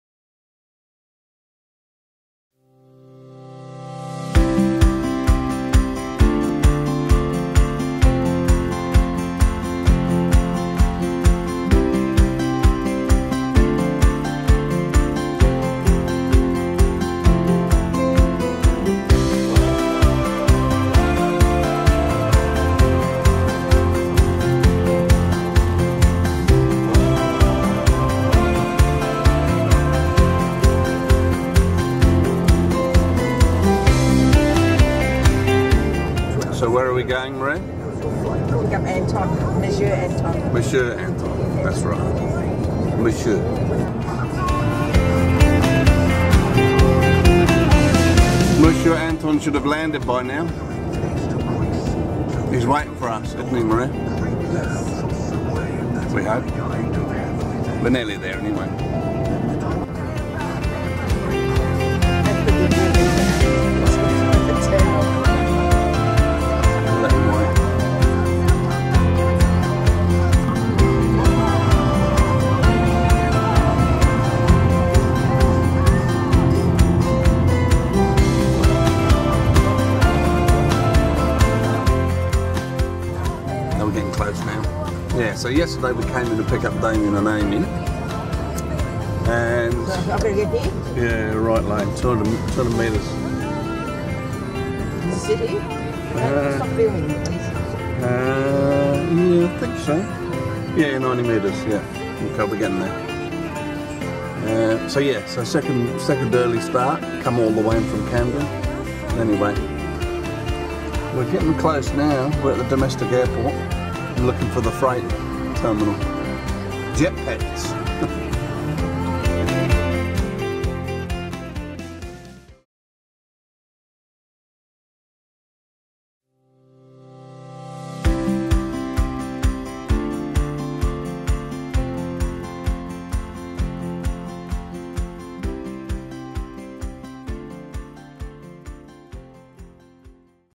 French Bulldog: Meeting Monsieur Anton Part 1
We go to Sydney Airport to pick up a French Bulldog called Anton. He has been living with his mother, an Australian Champion and his siblings in Perth WA. He will be starting a new life with us in Camden NSW. He is 11 months old as of April 2018.